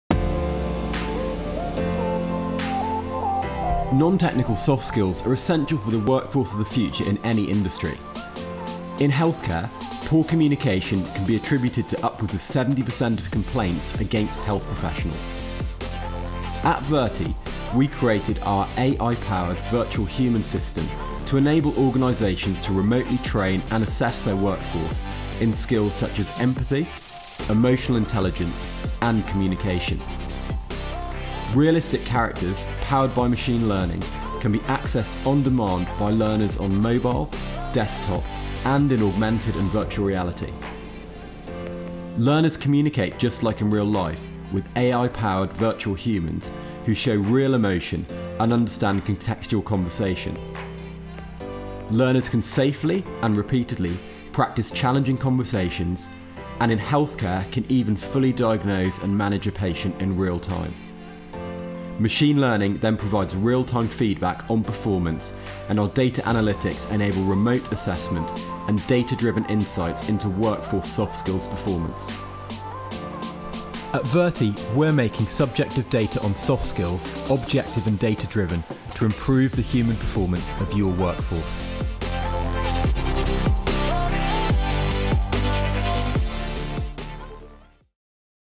Virti - Virtual Humans
Non-technical soft skills are essential for the workforce of the future, in any industry. At Virti, we've created an AI powered Virtual Human system, to enable organisations to remotely train and assess their workforce in these integral skills.